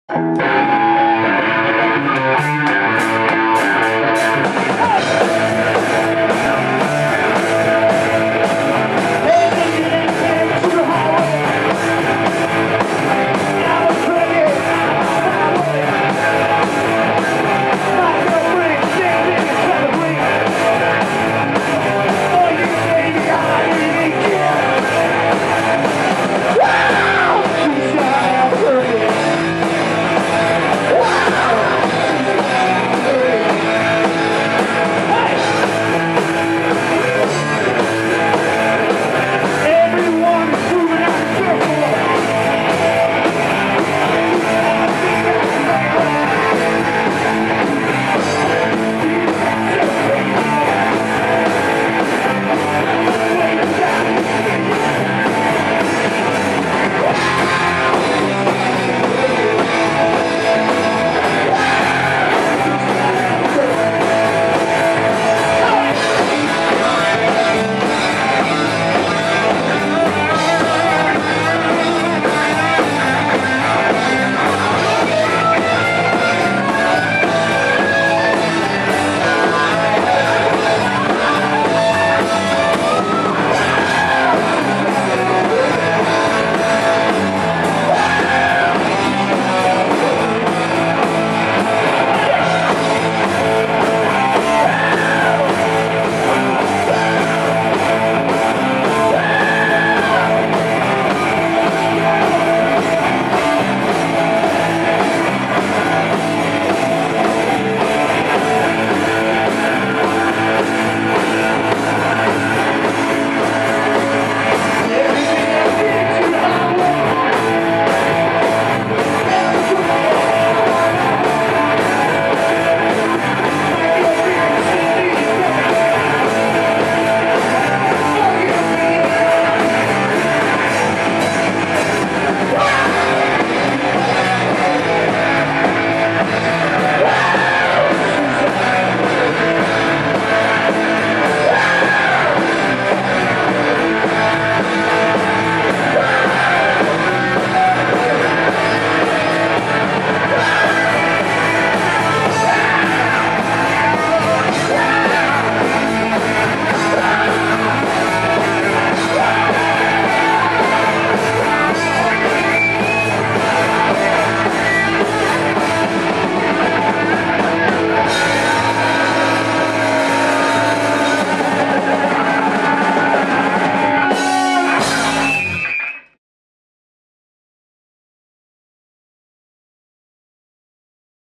Wild Scenes as Footage Leaks of Psychotic Turnbuckles Rehearsal
Prepare to meet your makers at the Bald Faced Stag in Lecihhardt on December 15.  Tickets for this Mexican Death Match are at moshtix.com.au